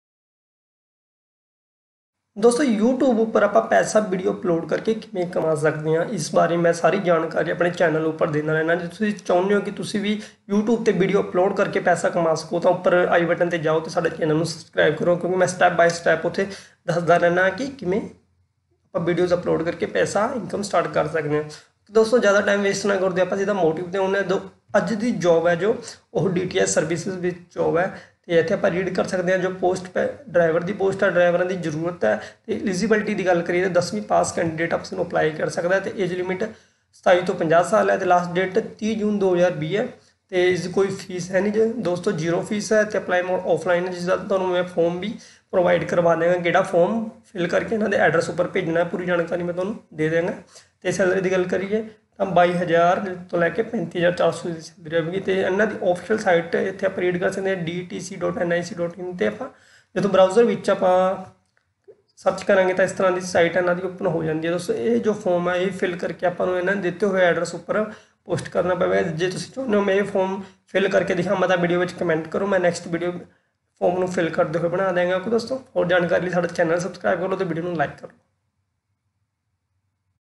Punjab Jobs 2020 Bus driver Jobs Vacancies for all India Candidate's
Hello Dosto This Is a Informative Channe.And This Video Is A job Vacancies Videos IN DTC Jobs Bus Driver Jobs !0th Pass Candidates Can apply Offline Jobs

Official Site  dtc.nic.in